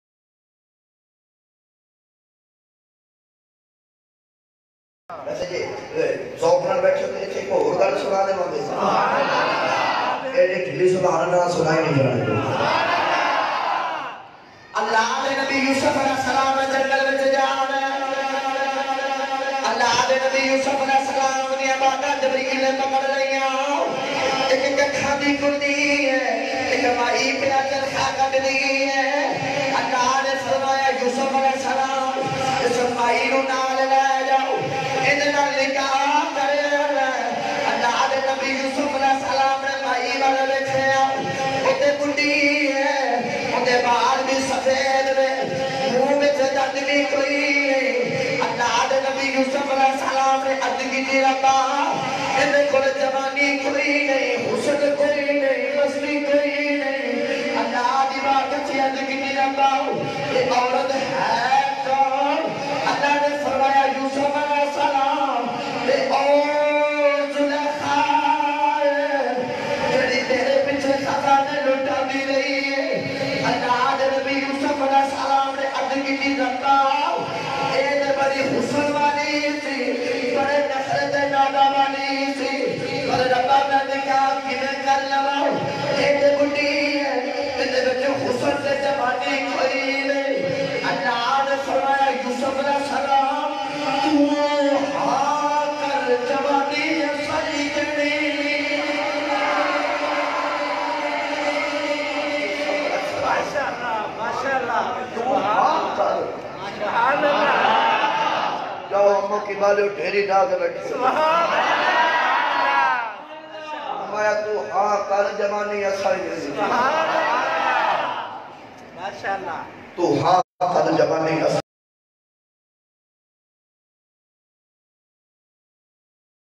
Allama Hafiz Sufyan Aasi Bayan _ Waqia Hazrat Yousuf a.s Aur Amma Zulekha _ Islamic History !! AS TV
AS TV*** کیا حضرت یوسف علیہ السّلام اور اماں زلیخہ کا آپس میں نکاح ہوا تھا۔۔۔۔۔۔دیکھئے ویڈیو علامہ حافظ سفیان آسی صاحب نے بہت ہی خوبصورت واقعہ بیان کیا ہے
      ***Ahle Sunnat Wa Jamat ki نمائیندگی krne wala youtube pr pehla channel . Ap ko iss channel pr tamam ikhtlafi masail ka hal mil sake ga.